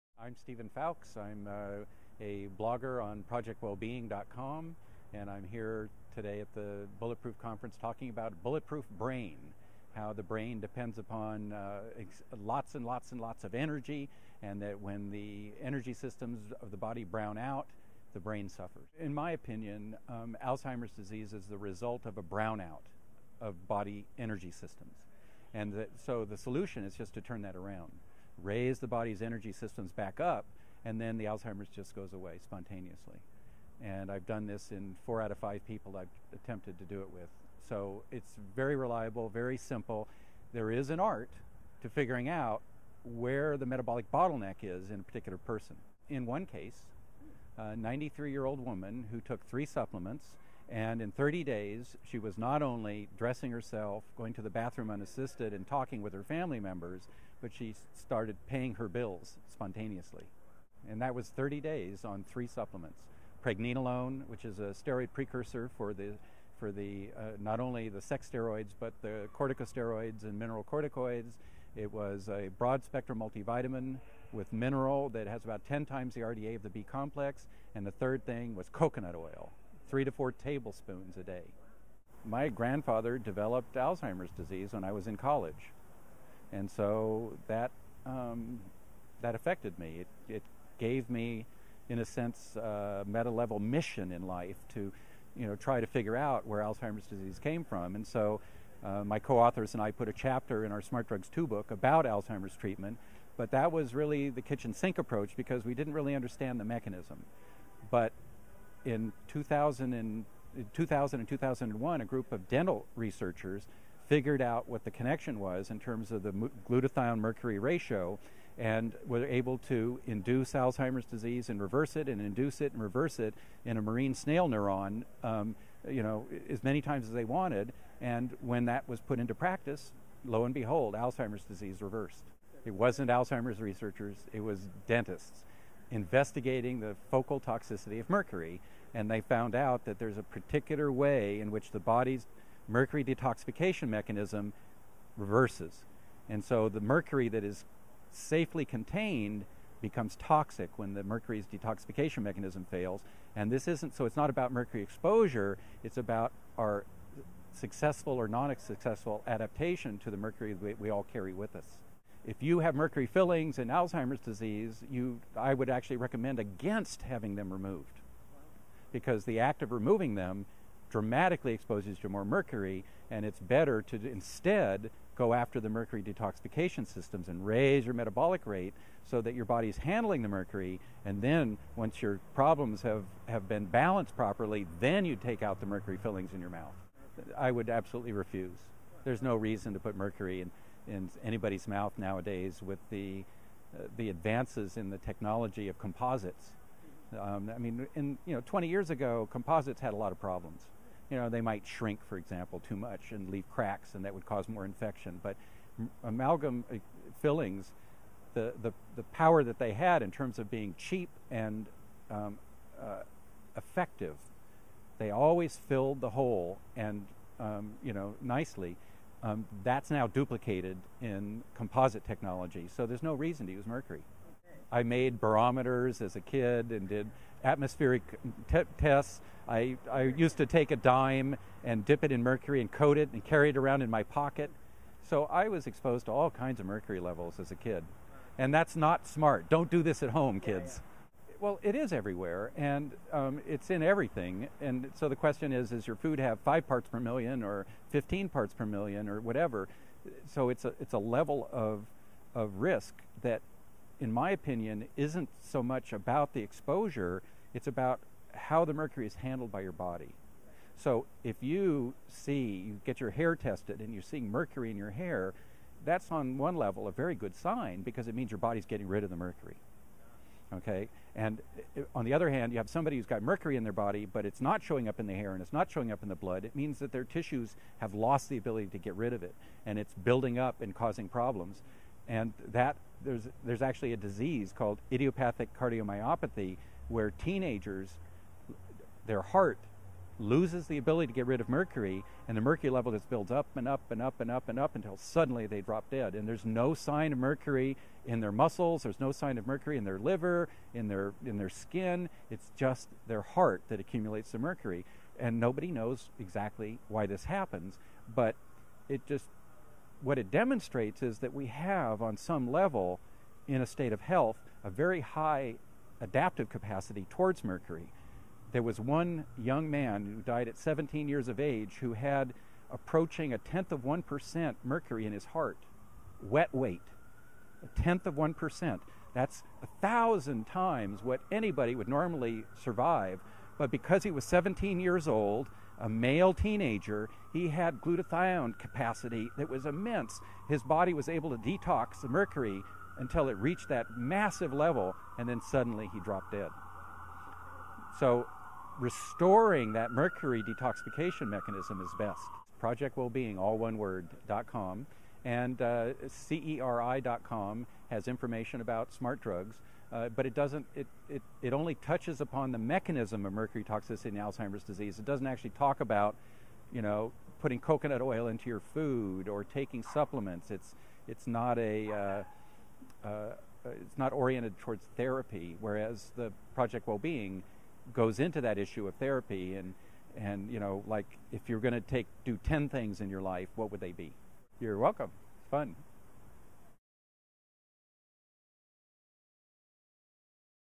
Biohacking Alzheimer's with Steve Fowkes
Nerd Stalker interview with Steve Fowkes of ProjectWellBeing.com at the Bulletproof Conference. We talk brain hacking to fight Alzheimers, Mercury levels and more.